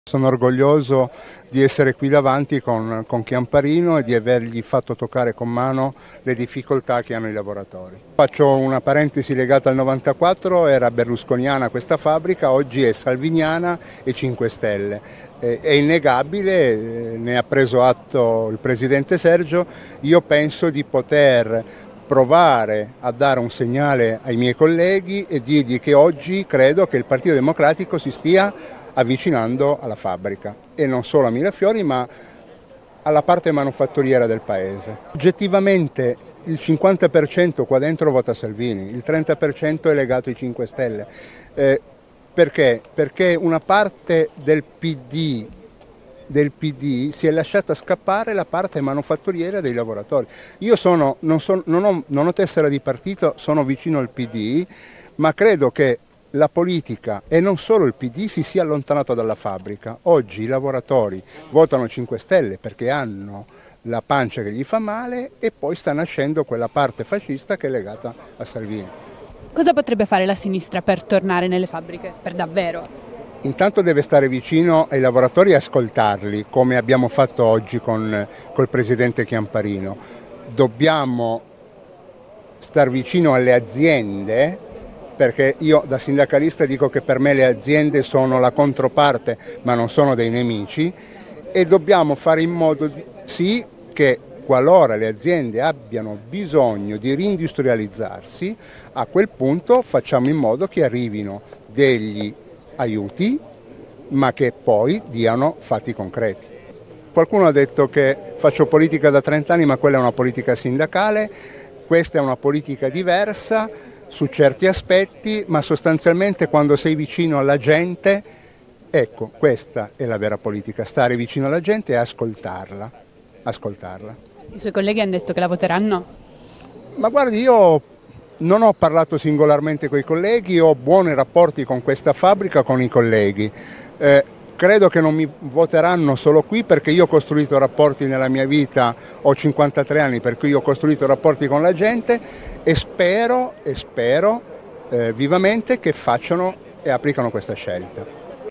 Piemonte, candidato operaio di Mirafiori: "Votano Salvini ma Pd è tornato in fabbrica"
Gianni Comparetto, candidato per i dem, ha portato Chiamparino all'uscita di Fca a Torino

POLITICA (Torino). "Sono orgoglioso di essere qui con Sergio Chiamparino e di avergli fatto toccare con mano i problemi degli operai - così Gianni Comparetto, operaio di Mirafiori candidato alle Regionali in Piemonte per il Pd - Nel 1994 questa fabbrica era berlusconiana, oggi è salviniana e 5 Stelle, io penso di poter provare a dare un segnale ai colleghi per dire che il Partito Democratico si sta avvicinando alla fabbrica. Il 50% qui dentro vota Salvini, il 30% il Movimento, perché il Pd se li è fatti scappare, la politica si è allontanata dalla fabbrica. La sinistra deve stare vicino ai lavoratori e ascoltarli, alle aziende, perché sono la controparte e non dei nemici". (Sara Iacomussi/alanews)